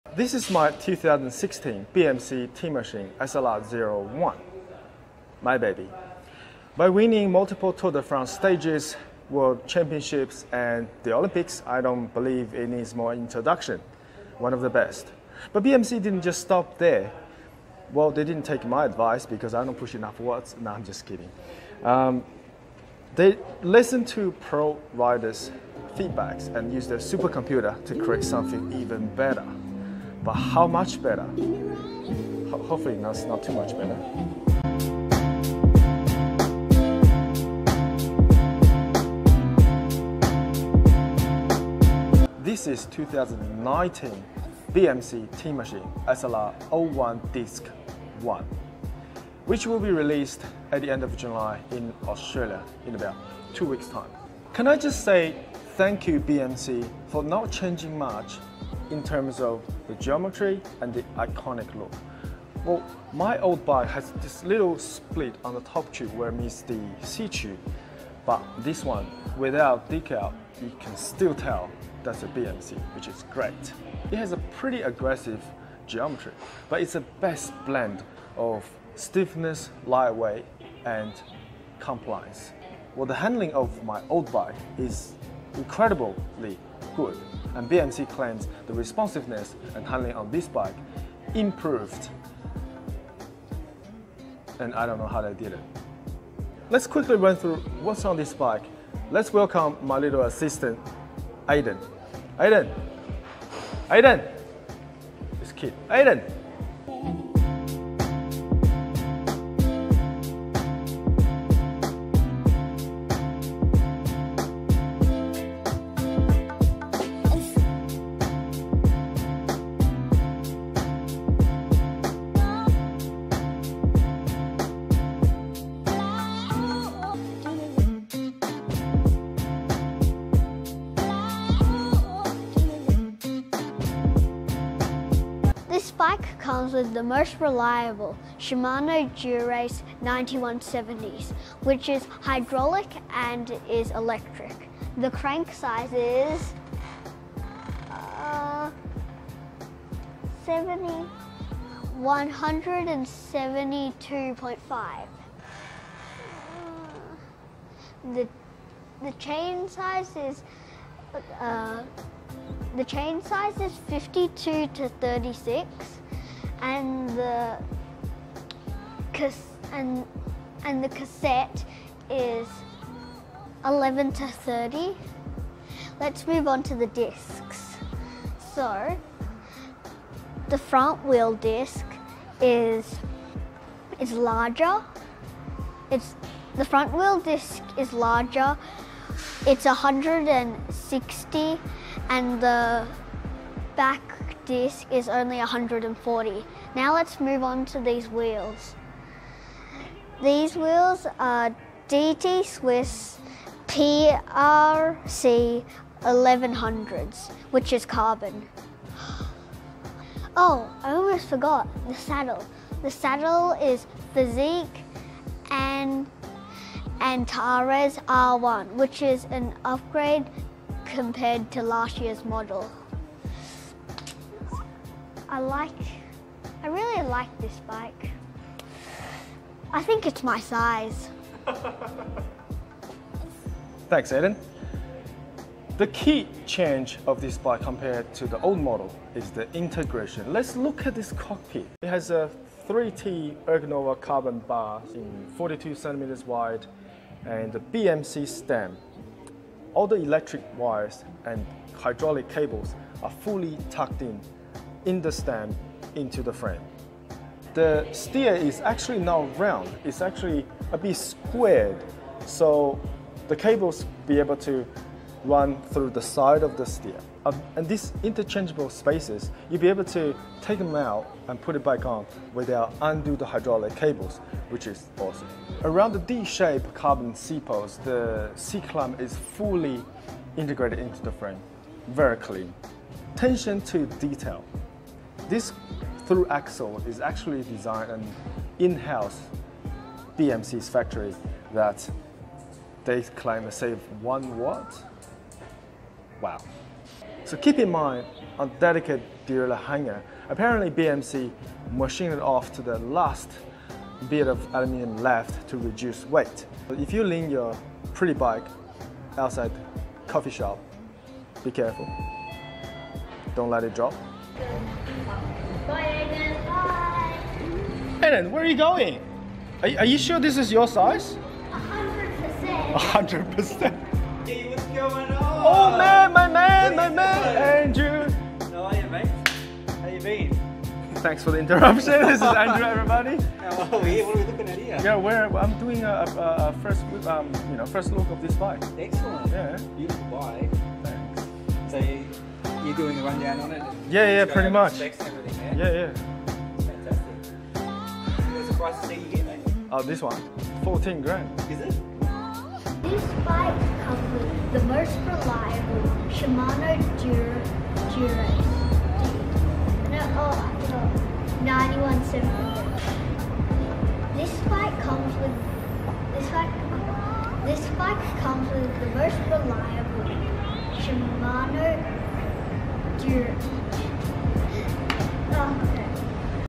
2019 BMC Teammachine SLR01 disc one first look | TDC BikeTech
Thanks for Bikeforce Docklands giving us the opportunity to have the first look of 2019 BMC Teammachine SLR01 Disc One before official release date.

Specs:
2019 BMC Teammachine SLR01 disc one
Shimano Dura Ace 9170 di2 groupset
DT Swiss PRC1100 wheelset thru axle
BMC stem
3T Ergonova handlebars
Fizik Anteras saddle